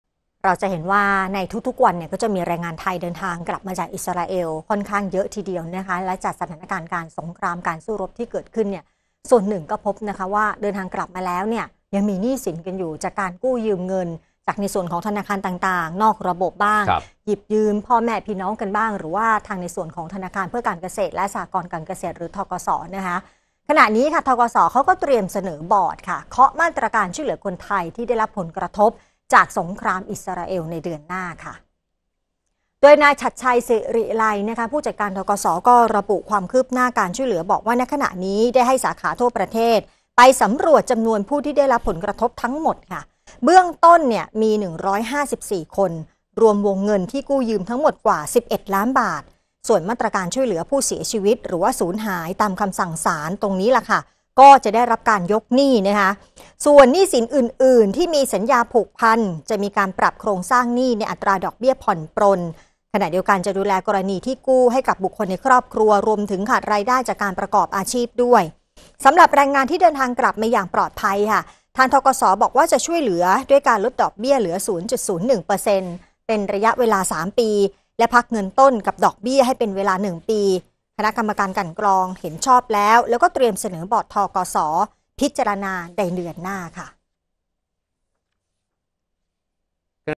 ธ.ก.ส. เตรียมพักหนี้ ช่วยแรงงานไทยจากอิสราเอล ข่าวเที่ยง วันที่ 24 ตุลาคม 2566
แรงงานไทยที่ทยอยเดินทางกลับประเทศอย่างต่อเนื่อง จากสถานการณ์สงครามในอิสราเอล ส่วนหนึ่งพบว่า มีหนี้สินจากการกู้ยืมเงินจากธนาคารเพื่อการเกษตรและสหกรณ์การเกษตร หรือ ธ.ก.ส. เพื่อไปทำงาน ขณะนี้ ธ.ก.ส. เตรียมเสนอบอร์ด เคาะมาตรการช่วยเหลือคนไทยที่ได้รับผลกระทบจากสงครามอิสราเอลเดือนหน้า